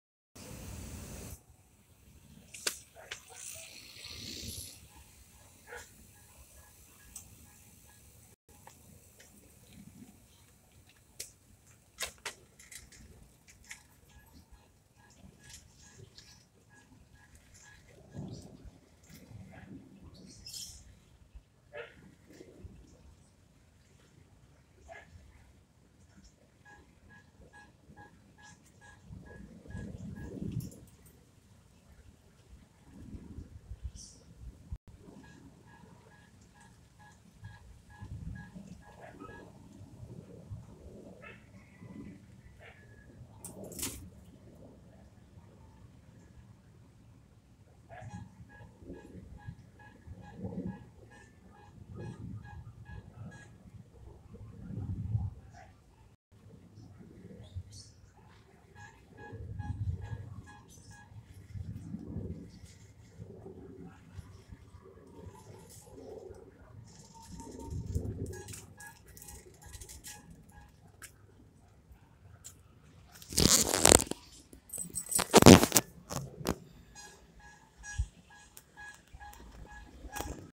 senior biking exercise
February 21 , 2022. Biking at Lentils , Robinsons homes east , Antipolo City